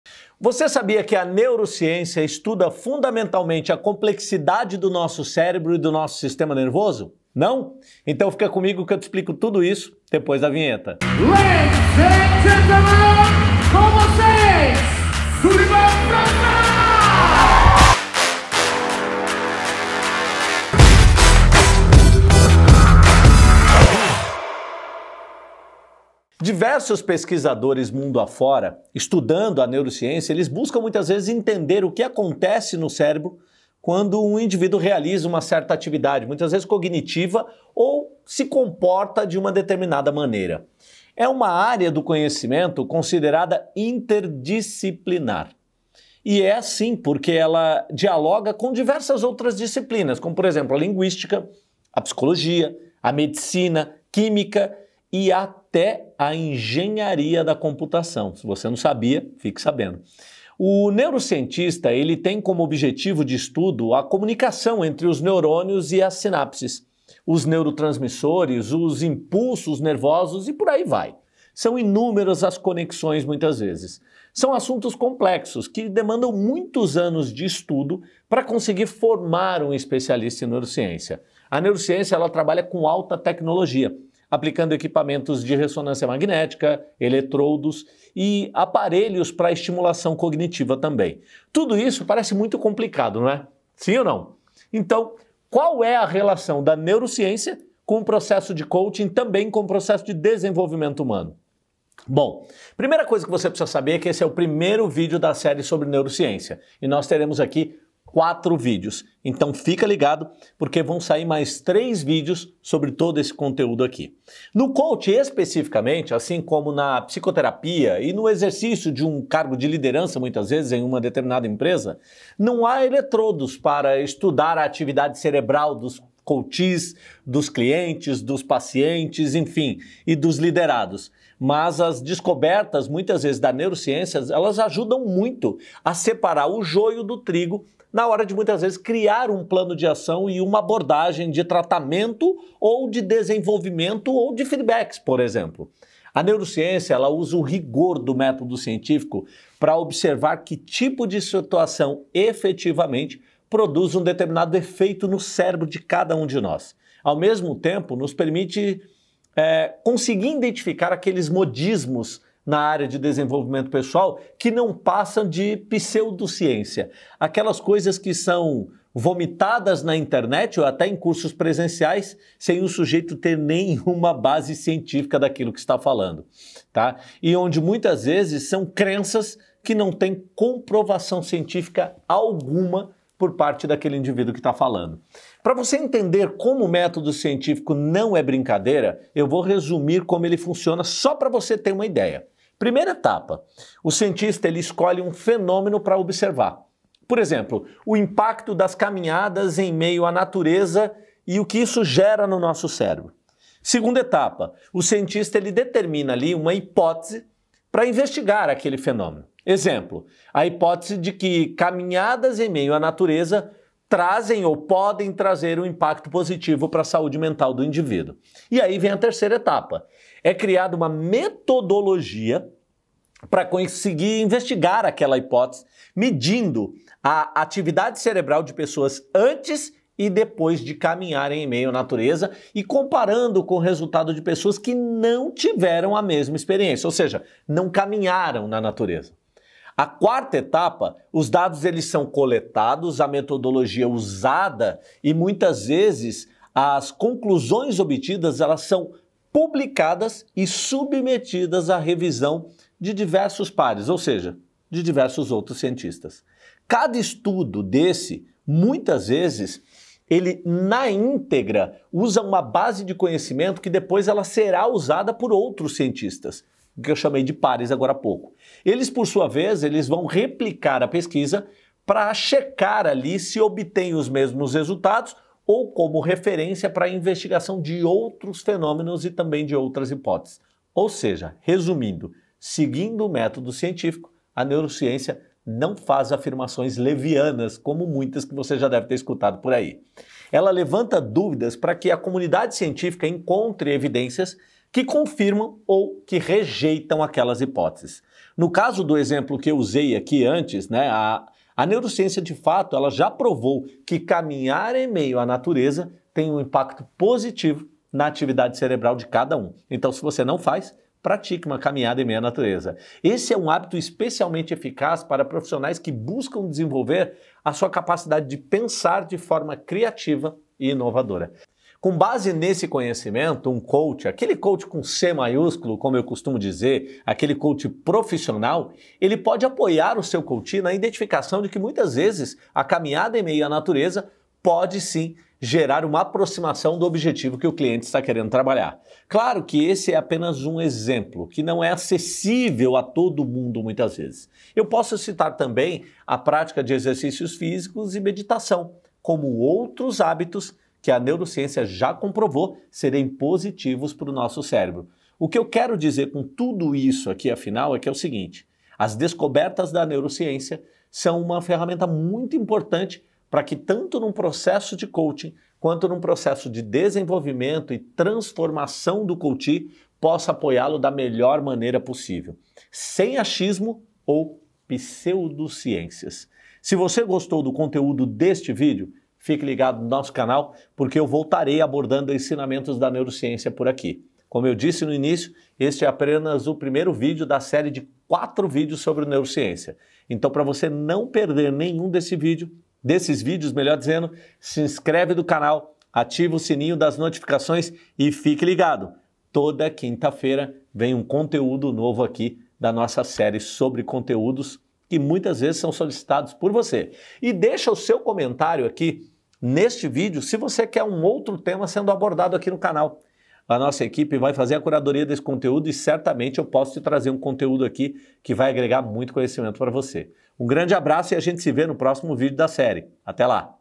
NEUROCIÊNCIA E A RELAÇÃO COM COACHING - Série Neurociência #01 | Sulivan França
Essa é a série "Neurociência" do Sulivan França. Neste episódio você vai aprender sobre a relação dessa ciência com o processo de Coaching e desenvolvimento humano.

Sulivan França - @sulivan_franca
Esse vídeo te ajudou de alguma maneira?